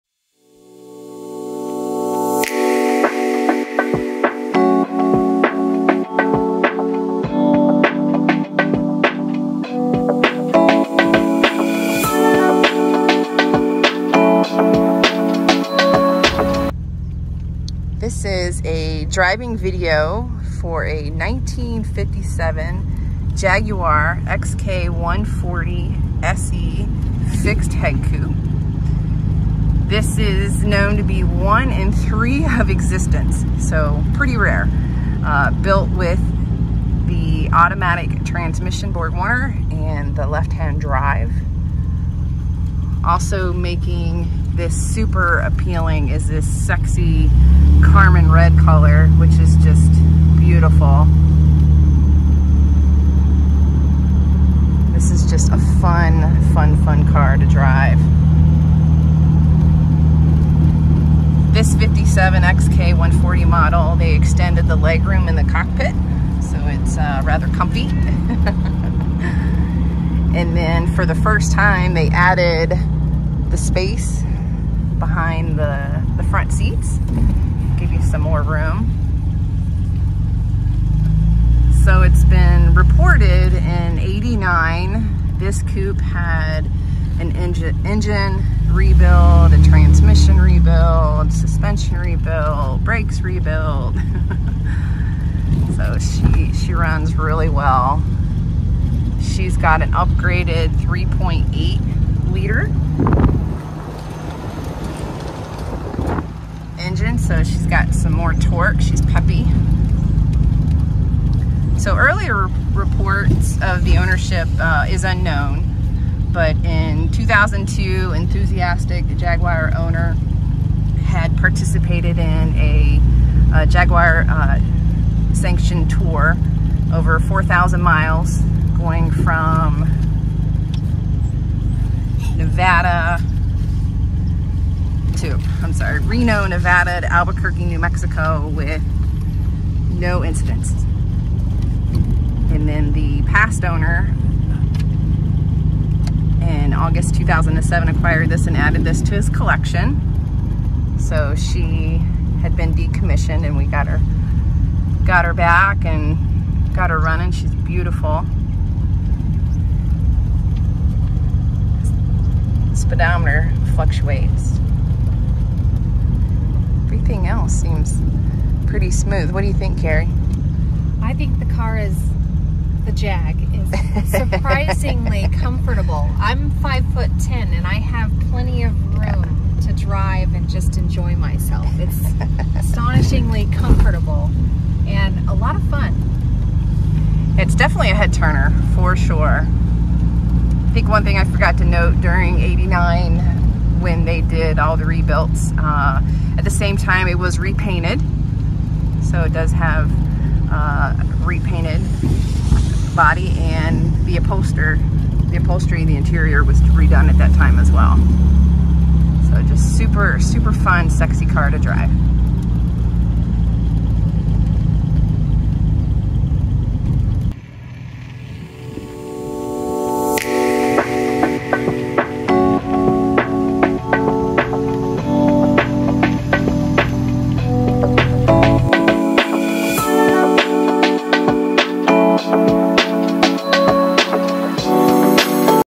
Test Drive - 1957 Jaguar XK140 SE Fixed Head Coupe!
This is a beautifully restored 1957 Jaguar XK140 SE Fixed Head Coupe that has accumulated 88,000 miles from new and is stunning in its Signal Red livery.  This Special Equipment (SE) Coupe is incredibly rare, reported to be one of only three built from new with left-hand drive and a 3-speed Borg Warner automatic transmission.  Having lived much of its life on the dry climates of Western US and then benefitting from a complete restoration, this SE Coupe is ready for concours exhibition or touring alike.

This XK140SE was completed at the factory on June 7, 1956 and was dispatched through Hornburg of Los Angeles to Harrah’s in Reno, Nevada, as confirmed by the Jaguar Heritage Trust Certification.   We could easily picture Bill Harrah driving this Jaguar as one of his new daily drivers.  The SE or also know as the MC, is the top-of-the-line offering with wire wheels, dual exhaust, fog lamps and a higher performance head from the C-Type.

Over the years, this SE Coupe has been a regular entrant at numerous shows throughout the 1990s and early 2000s, just after its restoration in 1989. This Jaguar was repainted and the interior upholstered while the drivetrain was out. It was also during this time that the engine was upgraded to a 3.8-liter block from a MkII with a head fitted with hardened valves and seats.  In 2002, the previous enthusiastic owner participated in a Jaguar sanctioned tour covering over 4,000 miles from Reno, Nevada to Albuquerque, New Mexico, all done without any incidents.  By 2007, this Jaguar was acquired and became part of a 3,500 car collection, one of the most valuable collections in the world.

The livery is sexy and sporty in its rare original Carmen Red that is in overall very nice show quality condition.  The paint has excellent shine and luster with only a few very small chips that have been touched up. The chrome overall is in very good to excellent condition.  We love the chrome wire wheels offering a perfect contrast to the Carmen Red paint.  The correct wide white wall tires are as specified on its build records.

The interior is stunning in its gray leather which remains soft and supple today.   The gray contrasts well and looks more contemporary today.  The gray carpet looks nice but shows some wear and use due to its history of touring.  The gorgeous burl walnut dash and trim are luxurious and elegant.  The wood was beautifully restored and looks very nice today.  In 1954 Jaguar extended the legroom of the interior and for the first-time added space behind the front seats.  This allows for storage of luggage while on tours.

This SE Coupe has a later model upgraded 3.8 liter DOHC engine with dual SU Carburetors delivering 220 bhp.  The three-speed Borg Warner automatic transmission with its upgraded engine make for ease of driving along with spirited performance.  The entire drivetrain was restored and rebuilt, including the engine, transmission, brakes, suspension, and rear end.  In our recommissioning of the SE Coupe, we went through the brakes, tuned and serviced it, as needed.  All of the instruments function well with only a wobble in the speedometer reading.  Overall, this Jaguar performs beautifully and is without issues.

The Jaguar comes with a file history, Jaguar Heritage Trust Certificate and includes a full spare, tools, and jack.  We love the stunning color combination and reliable drivetrain. This Coupe would make a terrific classic daily driver or weekend fun car.

We have this competitively priced.  The Hagerty No. 2 Value is $102,000 and the NADA Retail Value is $106,495.  We have this priced to sell.

For more information and details, please call, text or email Tina Smith at or .  It is available for inspection at our Phoenix showroom.